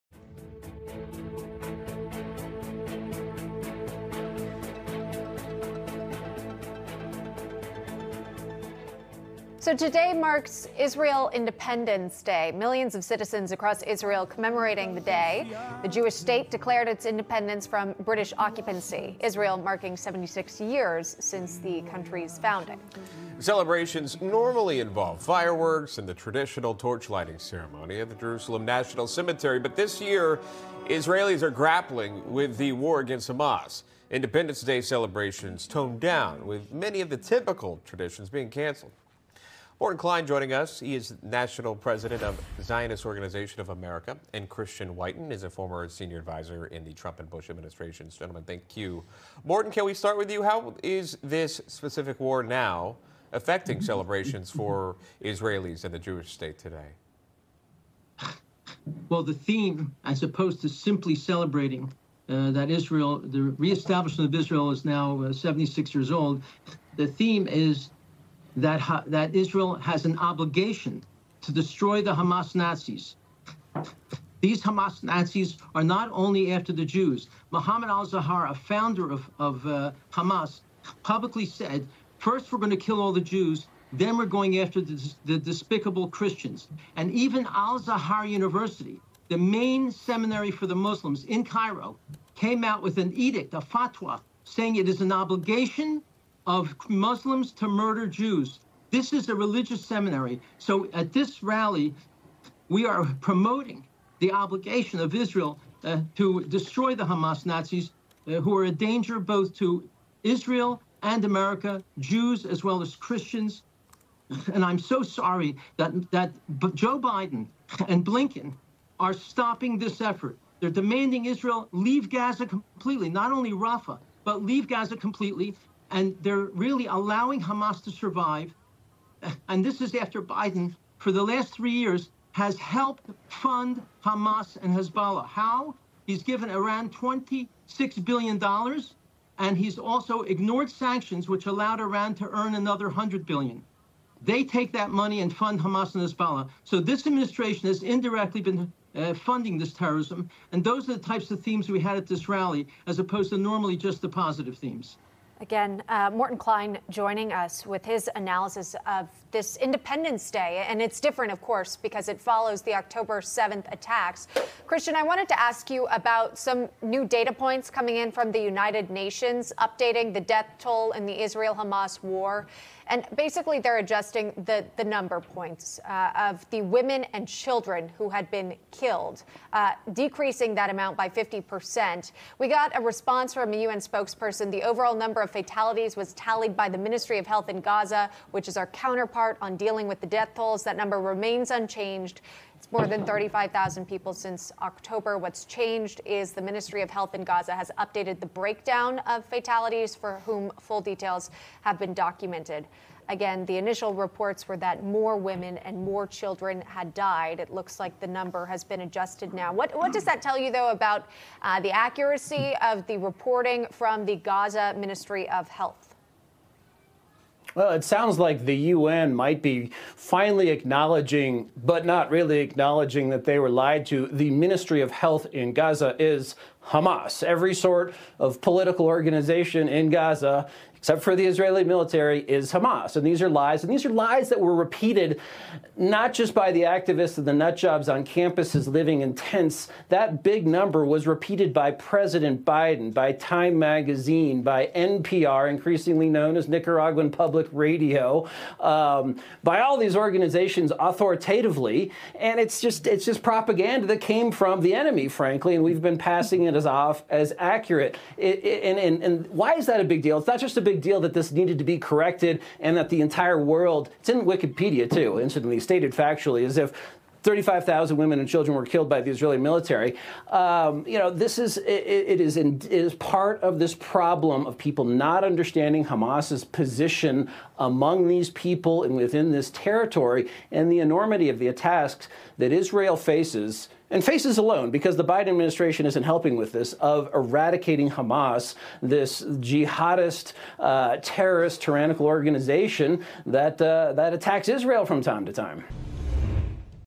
ZOA National President Morton A. Klein on Newsmax Discussing Biden Allowing Hamas to Survive
"They're demanding Israel leave Gaza completely...they're really allowing Hamas to survive."

Zionist Organization of America President Morton Klein and foreign policy analyst Christian Whiton slam the Biden administration's policy towards Israel.